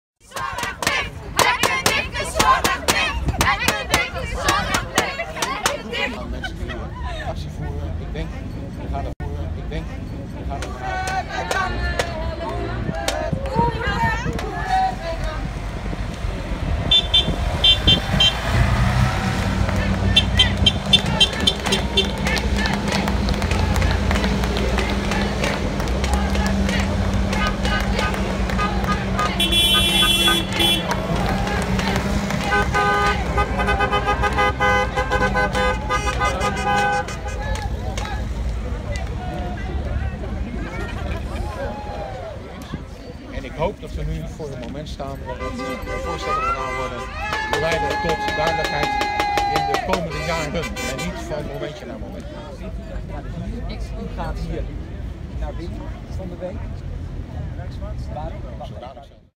Actievoerders Oostvaarderplassen bij Provinciehuis Flevoland
Actievoerders Oostvaardersplassen demonstreren ook bij Provinciehuis Flevoland. Compliment voor het gedrag van de actievoerders. Actie verliep zonder incidenten en in een goede sfeer.